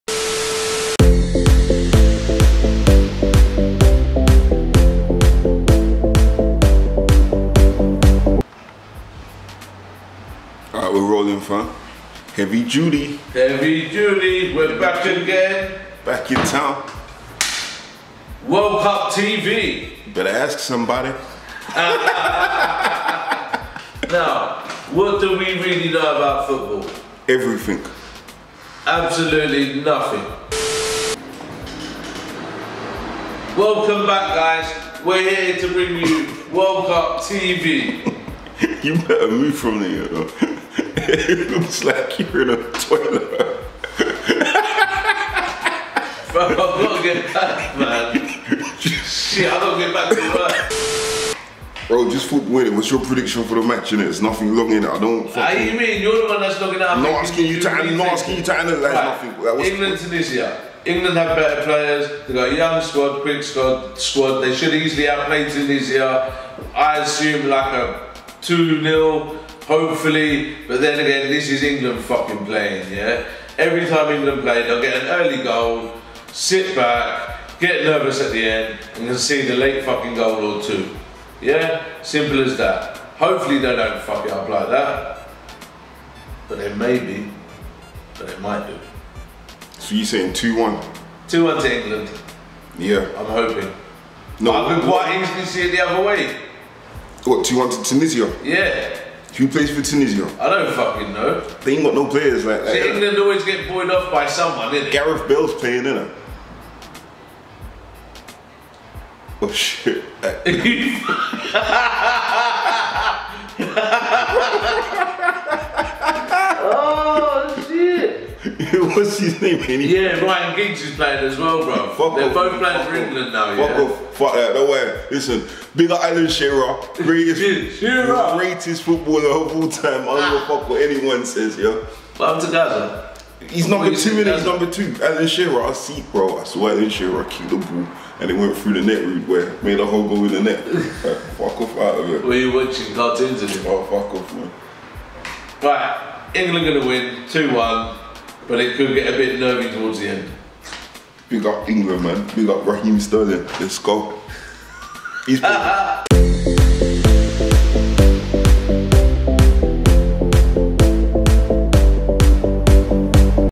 England vs Tunisia : Was Angry Shopkeepers Goal Prediction Right?
Former England Captain Angry Shopkeeper gives his prediction on England Vs Tunisia. The first England match of the 2018 World cup